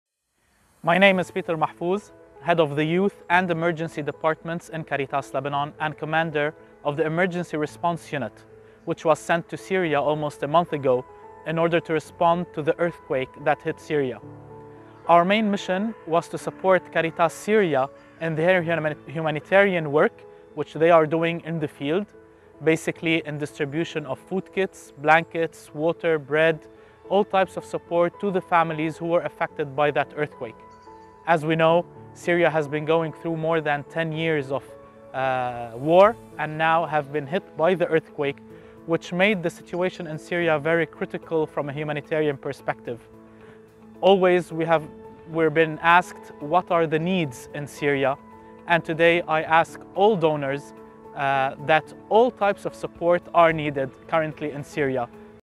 in syria. "Caritas Syria needs all the support that they can get"
After twelve years of war and the earthquake that hit Syria on February 6th, the country is in a very critical situation: "Caritas Syria needs all the support that they can get," says Peter Mahfouz, head of the Youth and Emergency Departments in Caritas Lebanon. Watch and share the video and please support Caritas' work in Syria